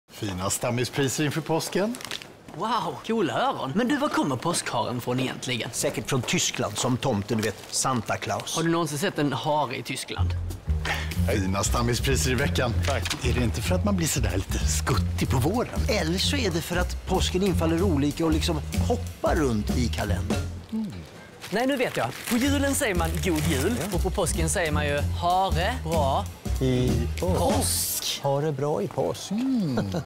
ICA reklamfilm v.12 2021 – Hare bra i påsk!
Var kommer egentligen påskharen ifrån?

Alla priser och erbjudanden gäller vecka 12 2021